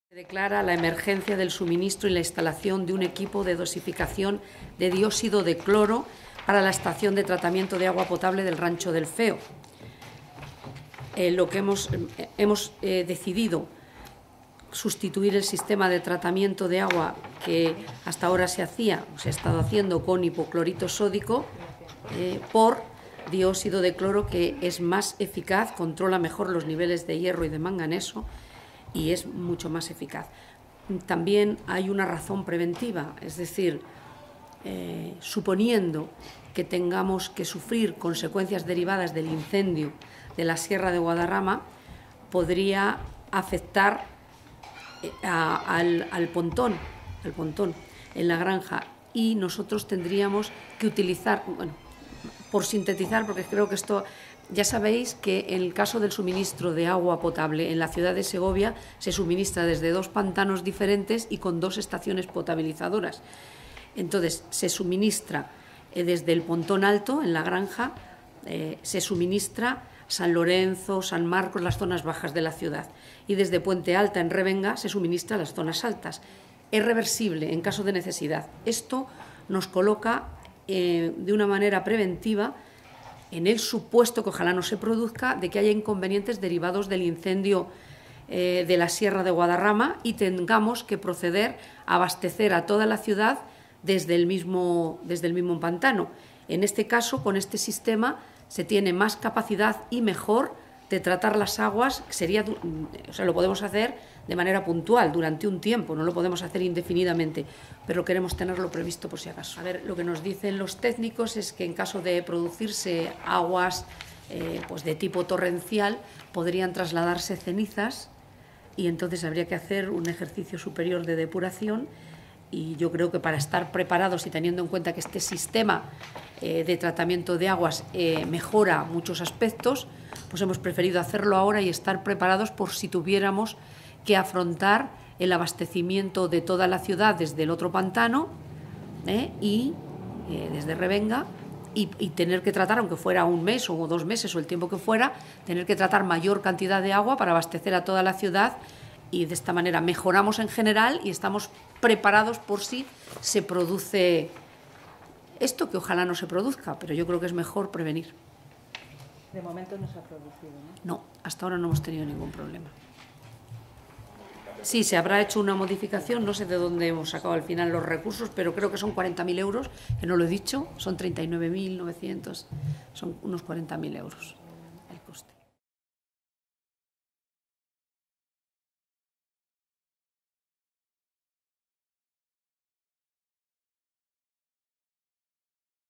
Casi 40.000€ de emergencia, por si las cenizas de Guadarrama afectan al agua del Pontón
El Ayuntamiento de Segovia ha aprobado destinar 40.000 euros para controlar los niveles de hierro y manganeso del agua potable de la ciudad, en prevención de que las cenizas del reciente incendio de la Sierra de Guadarrama pudieran afectar al Pontón Alto, según ha indicado este jueves la alcaldesa, Clara Luquero. La empresa adjudicataria es Aquona (nombre que adopta Aguas de Barcelona en Castilla y León).